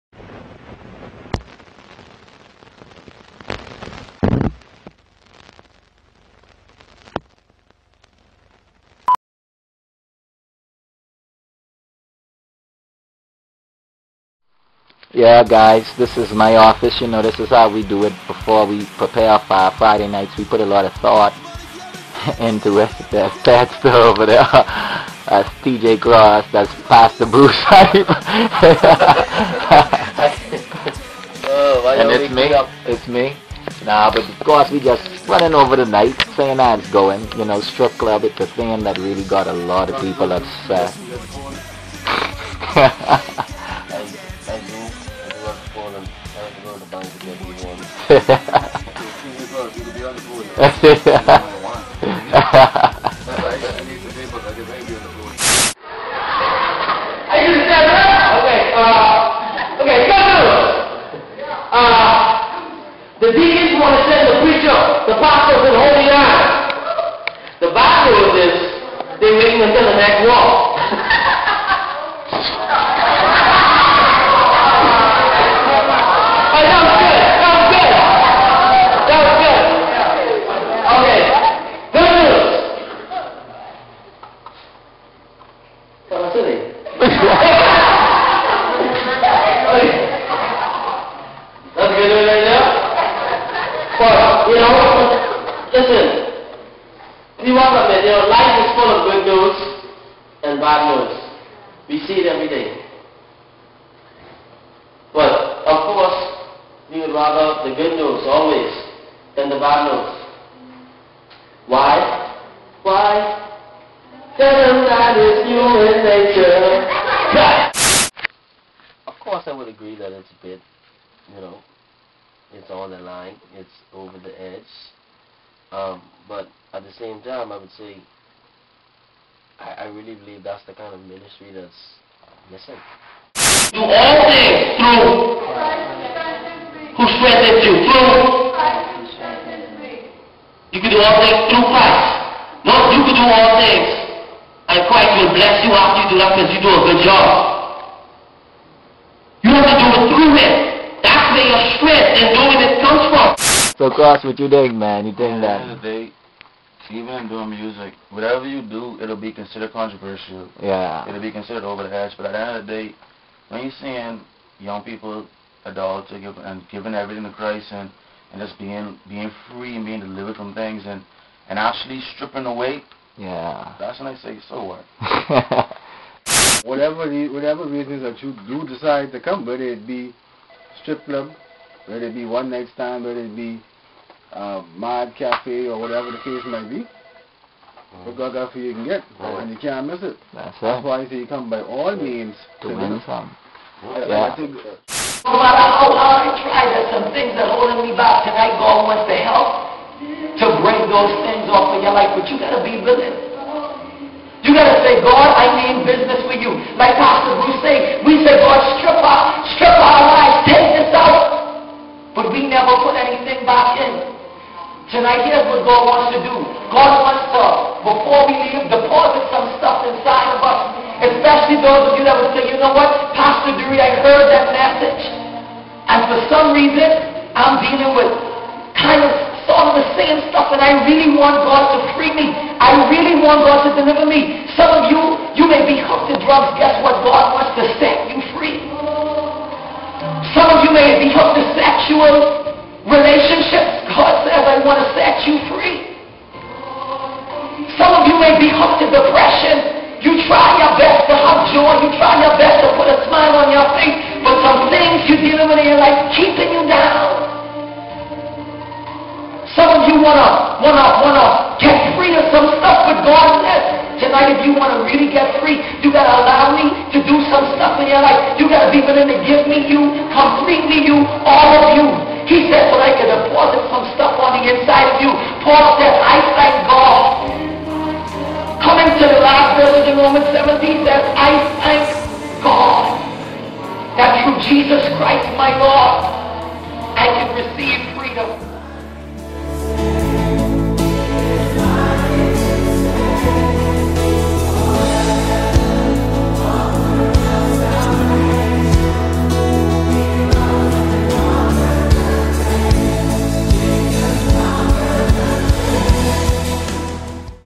MAD Fridays: Stripclub
Go behind the scenes of MAD Fridays February series "Stripclub" with the team.